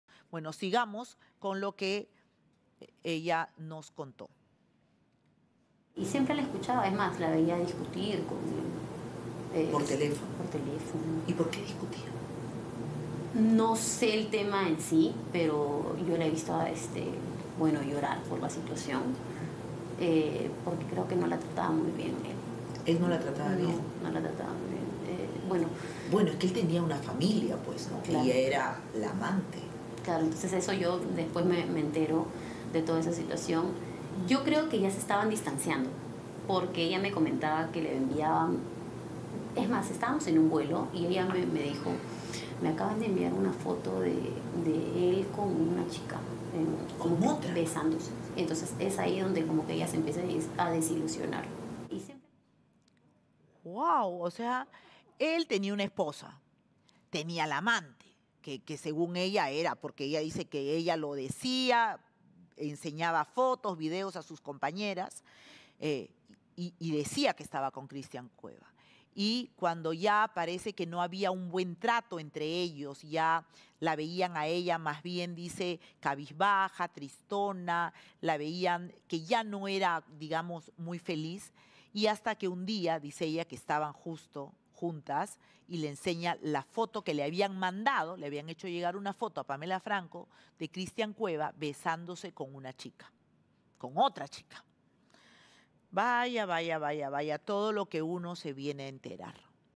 Excompañera de Pamela Franco sobre Christian Cueva: "La vi llorar porque él no la trataba muy bien"
Estrella Bereche incluso menciona que presuntamente habría existido una tercera mujer en toda esta situación.